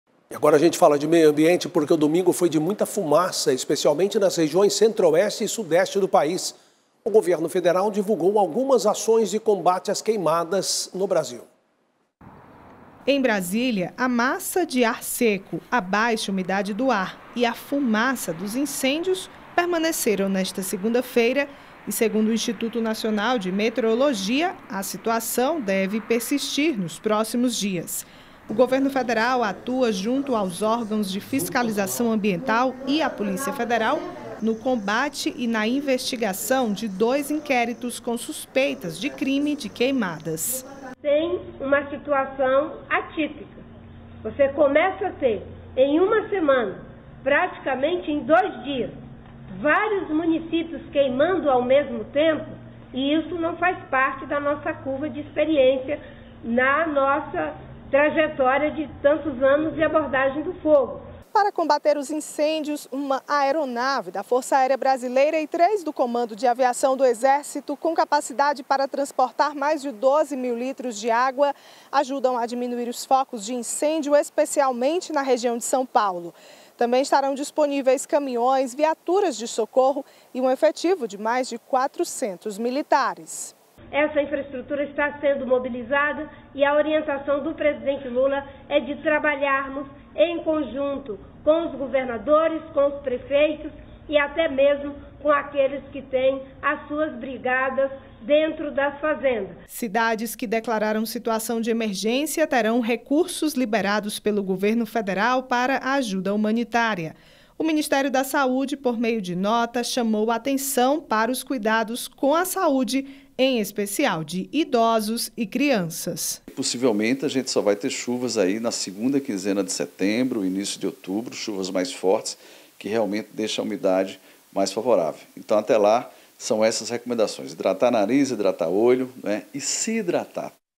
Governo Federal anuncia ações de combate as queimadas
O domingo foi de muita fumaça, especialmente nas regiões centro-oeste e sudeste. O governo federal divulgou algumas ações de combate as queimadas no país.

Reportagem de Liana Nunes e Ersomar Ribeiro

Deus abençoe!